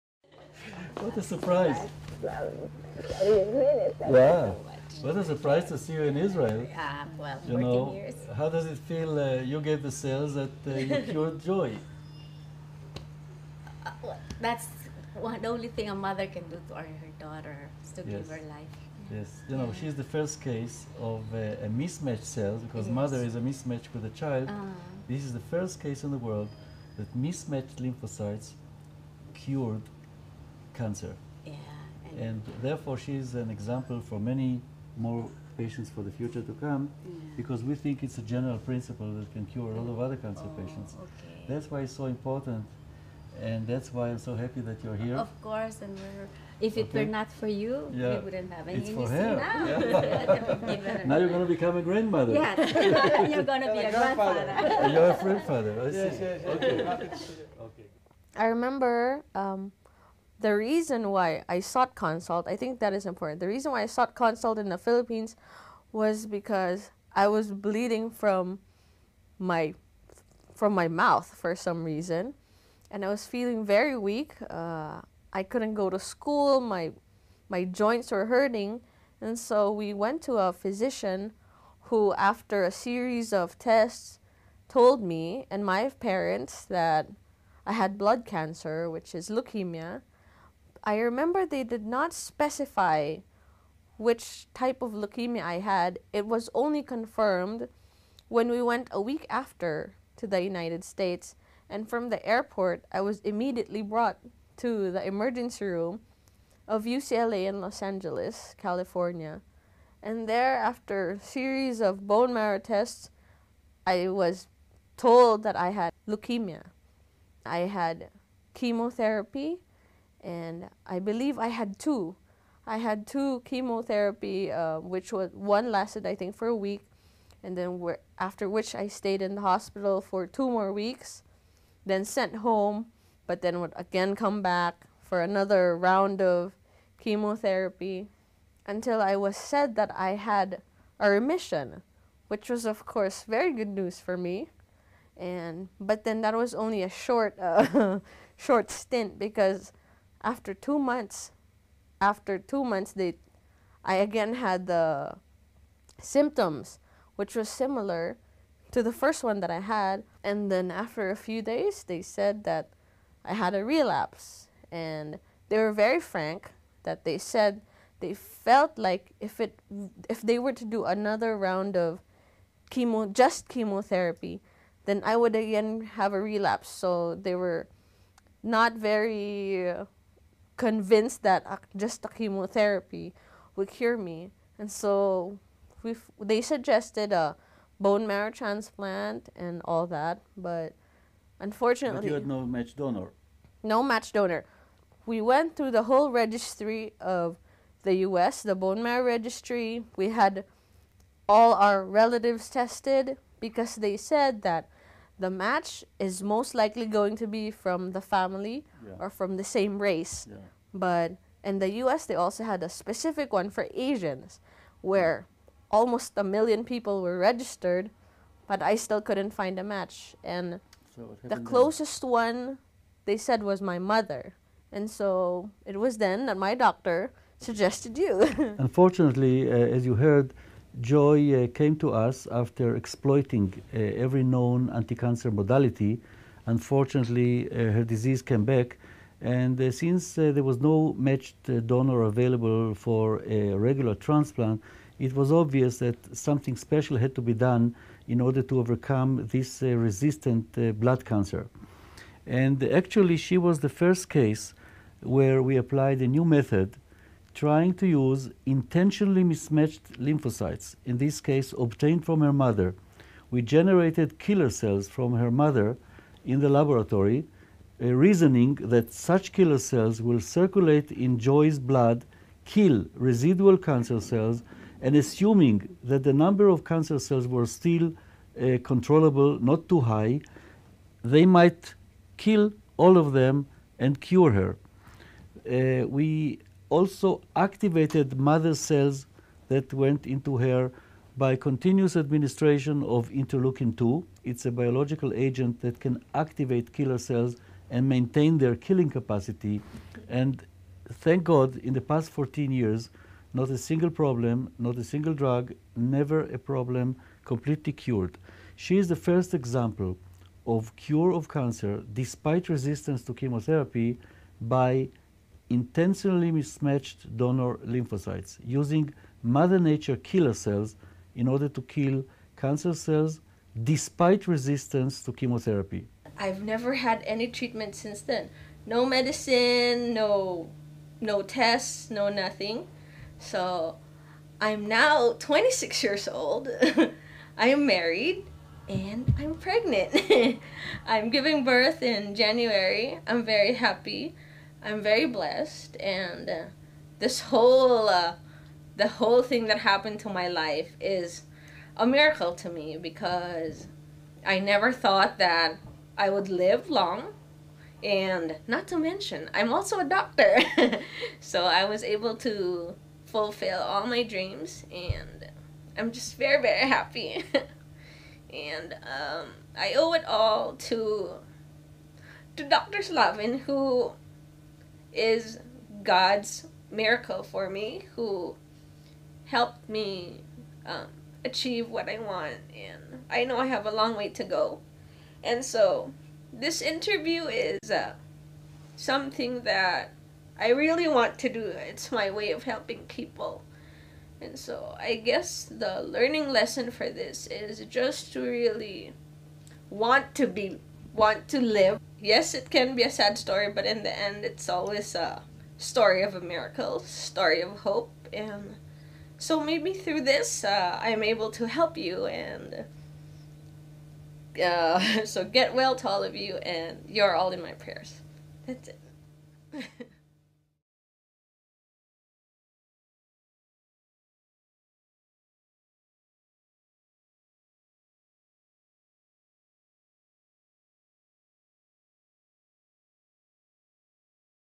Первый в мире случай полного излечения от рака
Джой — первая в мире пациентка, которая полностью излечилась от рецидива рака с помощью инновационной технологии лечения клетками-киллерами рака. Впервые в истории было проведено лечение несовместимыми лимфоцитами донора и их активация при помощи Интерлейкина -2.
С момента лечения прошло уже более 25 лет. Спустя несколько лет после лечения Джой родила ребенка и получила профессию врача. Лечение проводил всемирно известный онколог, профессор Шимон Славин